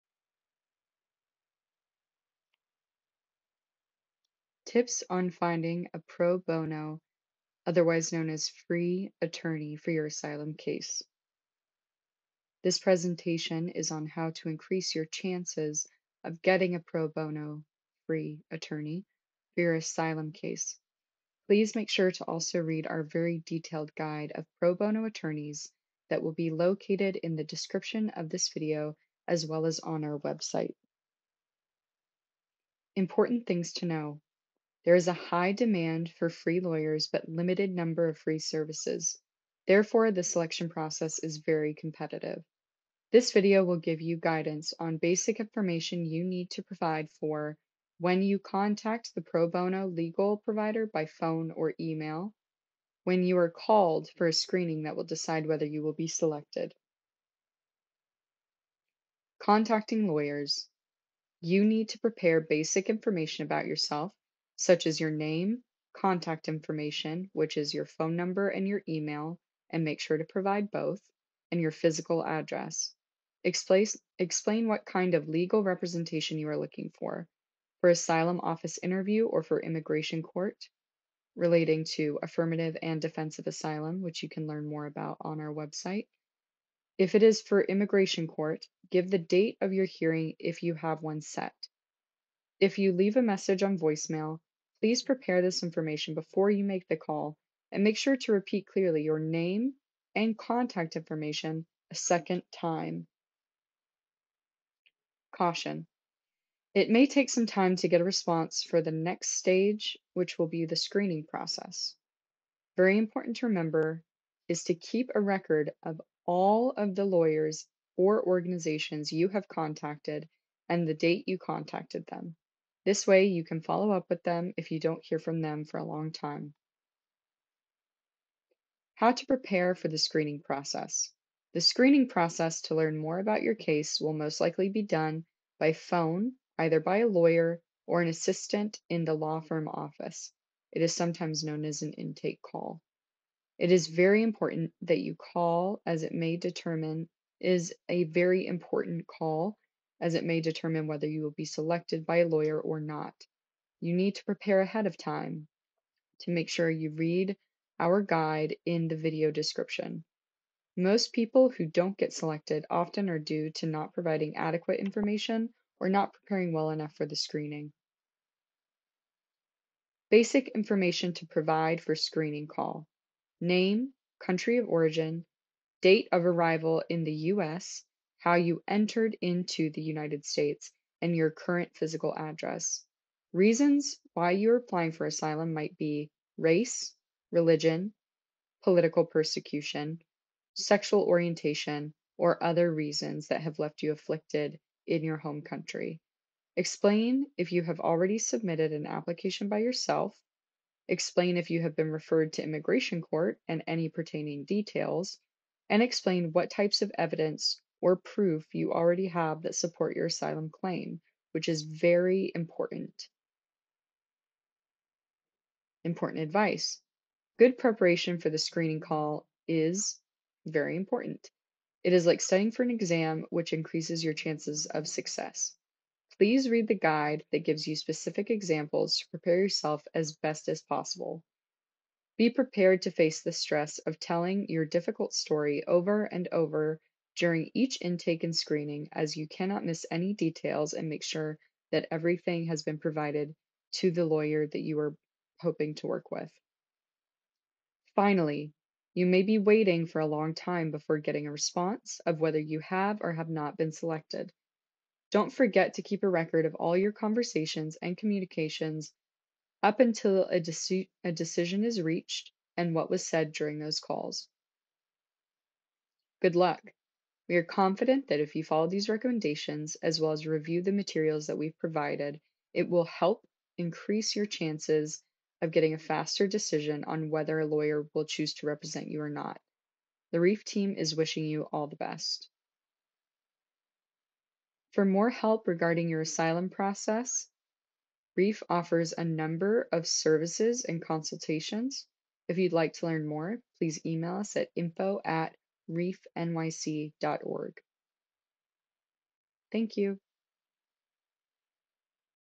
Tips on finding a Pro Bono (free) Attorney for your Asylum Case - RIF Asylum Support
Produce by the RIF Legal & Administrative Team

This is a brief overview of what to expect and how to prepare when contacting Pro Bono (free) Attorneys to represent you in your Asylum process.

RIF Asylum Support is a NYC-based organization dedicated to helping asylum seekers.

If you have more questions about the Asylum Process or would like to ask about other legal options, please join us for our virtual Weekly Asylum Info Session, Every Tuesday from 12pm-1pm (NYC Time). To find the zoom link to join,

RIF Asylum Support offers information and resources for Asylum Seekers.